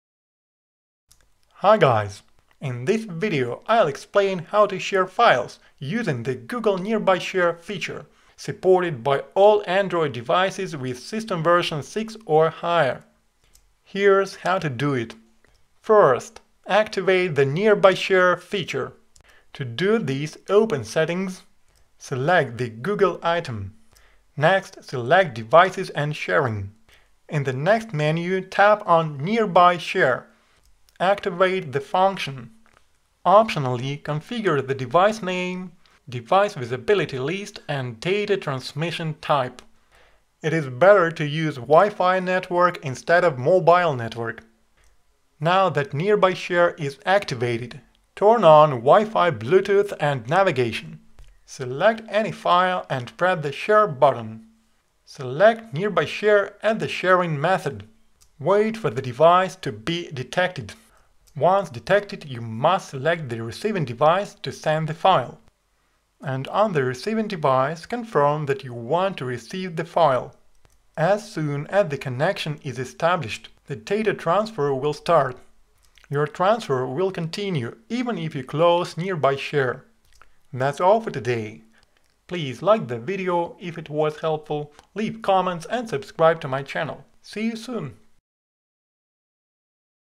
How to Use Google Nearby Share
Here I've shown how to use Google Nearby Share on an android phone. I've explained how to share files using the Google Nearby Share feature, supported by all Android devices with system version 6 or higher.

To use Google Nearby Share follow these steps:

- Activate the Nearby Share feature. To do this, open **settings**.
- Select the Google item.
- Next, select Devices & sharing.
- In the next menu, tap on Nearby Share.
- Activate the function. Optionally configure the device name (Device name), Device visibility - list and data transmission type. It is better to use Wi-Fi network instead of mobile network.
- Now that Nearby Share is activated, turn on Wi-Fi, Bluetooth and navigation.
- Select any file and press the "share" button.
- Select Nearby Share as the sharing method.
- Wait for the device to be detected.
- On the receiving device, Nearby Share, Wi-Fi, Bluetooth and navigation must also be activated.
- Once detected, you must select the receiving device to send the file. And on the receiving device, confirm that you want to receive the file.
- As soon as the connection is established, the data transfer will start. Your transfer will continue even if you close Nearby Share

Here you will find videos about using and optimizing your android phone. In addition, I make reviews of android apps and cell phones.